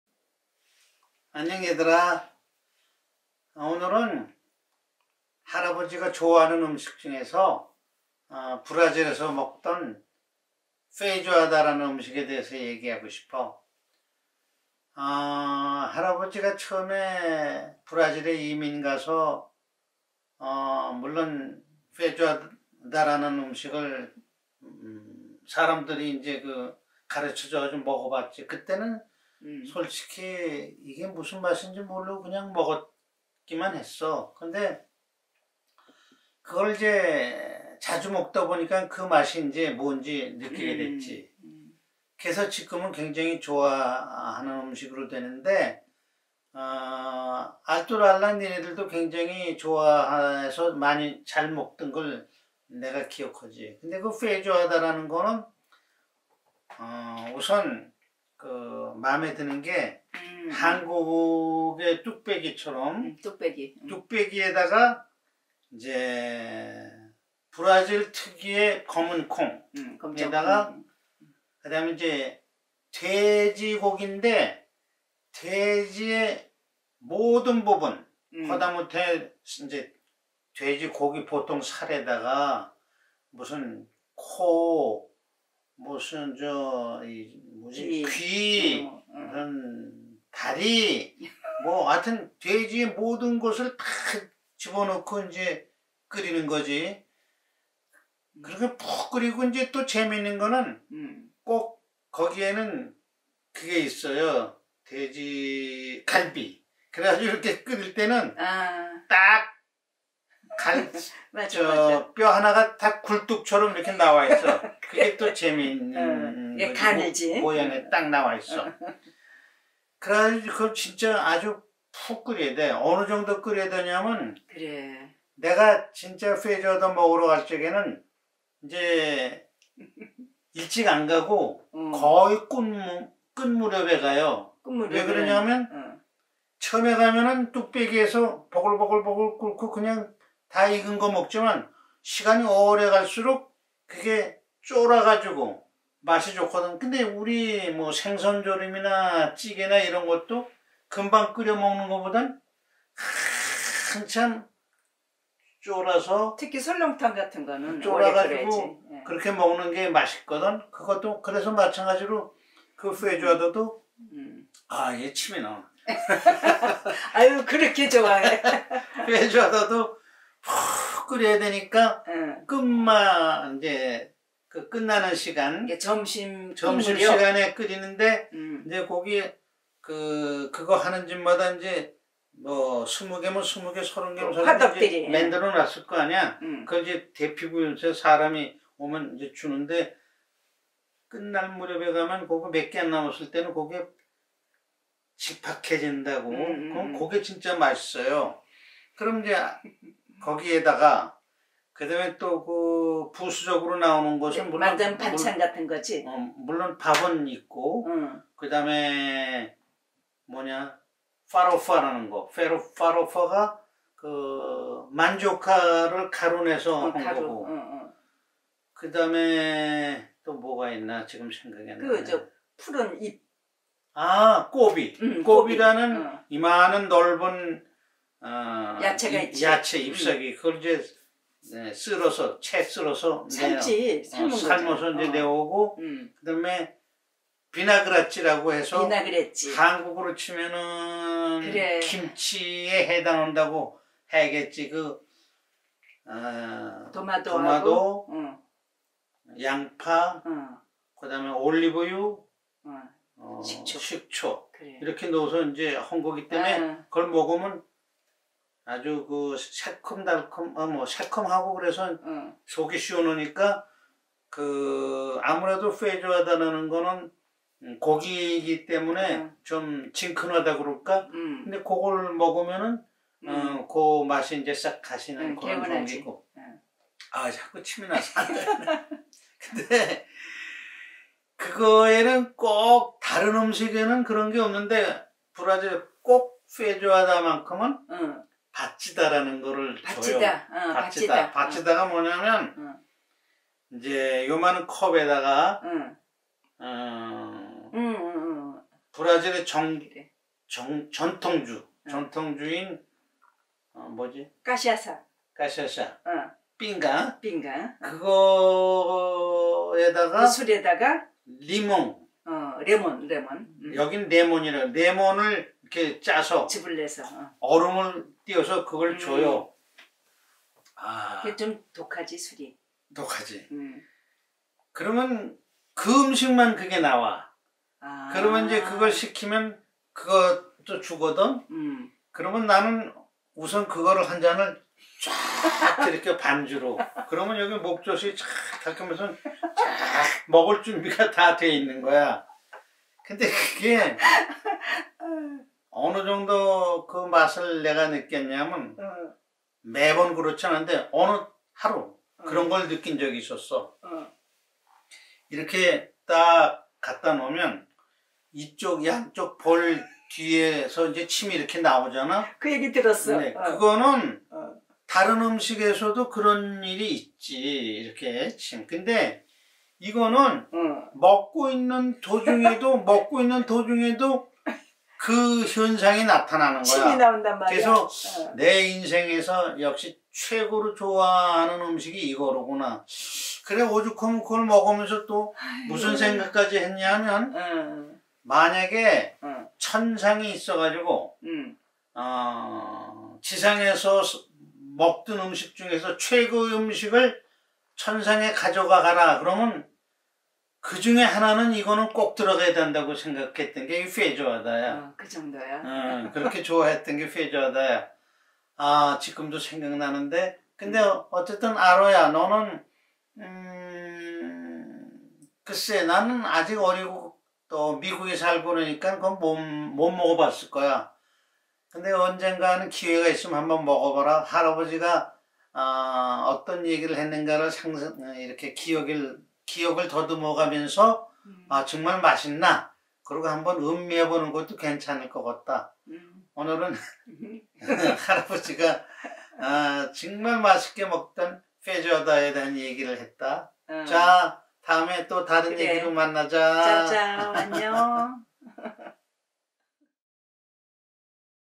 Dear Grandchildren - Feijoada!
Do you know the food called feijoada? This amazing food that anyone who has traveled to Brazil will never forget! This food called feijoada, which can be found anywhere, is a healthy food for Brazilians.
Oh, feijoada, it makes salivate just by thinking about it.

‘훼이조아다’란 이름의 음식을 아십니까? 한번 브라질을 여행온 사람은 결코 잊을 수 없다고 하는 이 놀라운 음식! 어느 곳에 가서도 만날 수 있는 이 훼이조아다는 브라질 사람들에겐 일상의 음식이면서도 보양식입니다.
아, 생각만해도 침이 고이는 ‘훼이조아다’